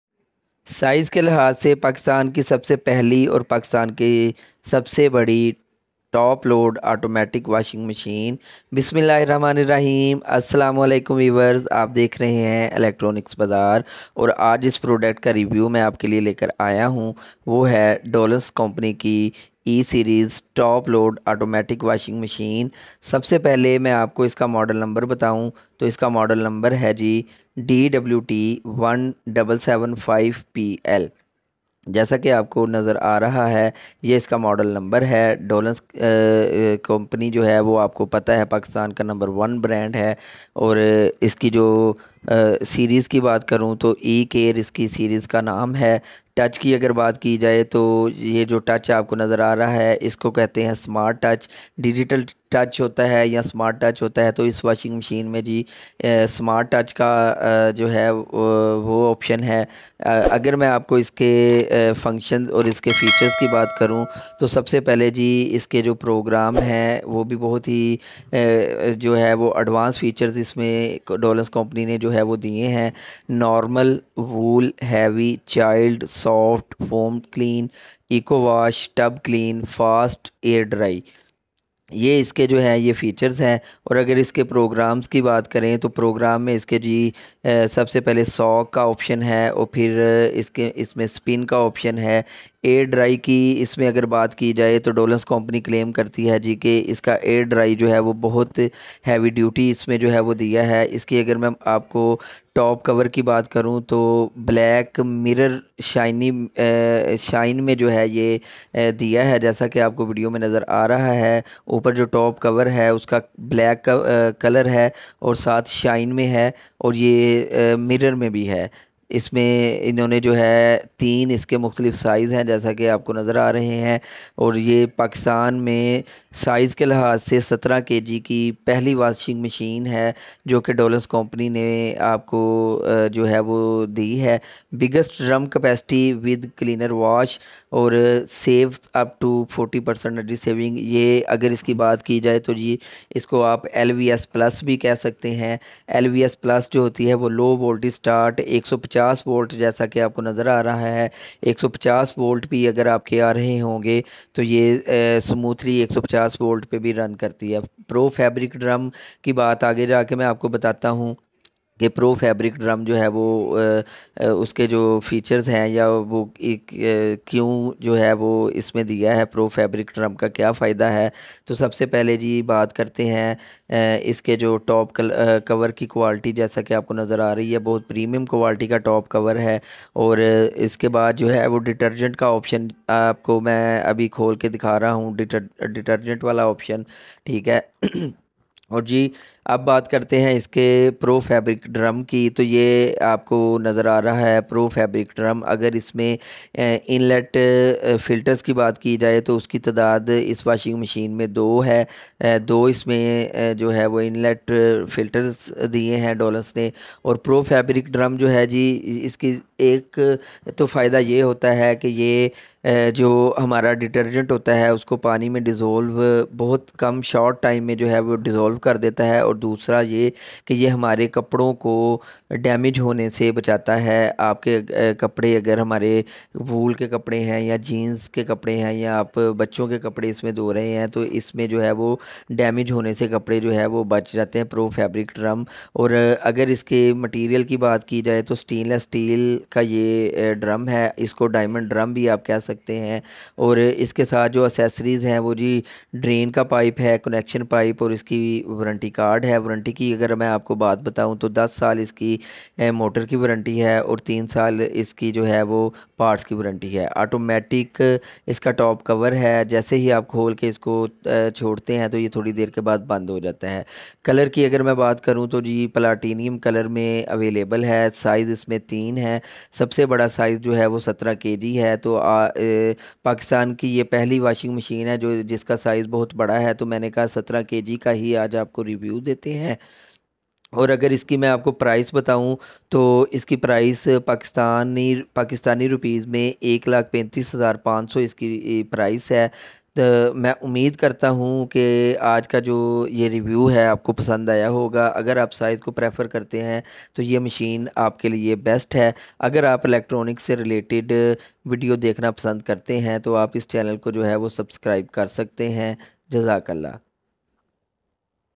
Dawlance Top Load Automatic washing machine 17Kg || DWM1775PL || 2023
Rs.139,500

Energy Saving
The topload range allows saving of at least 38% energy when using Eco program.

Air Dry
Say bye to your old, smelly washing machine and get the best Topload washing machine on the market! With maximum whirling power, our machines will get your clothes cleaner and dry them for as long as 90 minutes.

Pro-Fabric Diamond Drum
Enhance wash quality with an efficient and a fabric-friendly drum design: It ensures easy dissolving of detergents and provides better wash performance.

LVS+
The Low Voltage technology allows washing operation at as low as 150 volts, without the need for investing in additional cost of stabilizers, ensuring high quality washing performance everytime.

Energy Information
Voltage (V)
220
Frequency (Hz)
50

Features
EZ Wash (One Touch)
Yes
Air Dry
Yes
Child Lock
Yes
Buzzer
Yes
Functions
Programs
10
Function 1
Energy Saver
Function 2
Soaking
Function 3
Low Noise
Water Inlet
Single
Wash Programs
Program 1
Normal Fuzzy
Program 2
Air Dry
Program 3
Heavy Fuzzy
Program 4
Soft
Program 5
Eco
Program 6
Fast
Program 7
Wool
Program 8
Child
Program 9
Foam Clean
Program 10
Tub Clean
Design & Outlook
Color
Platinum
Drum Pattern
Diamond
LED Display Color
Green
Performance
Motor Type
Induction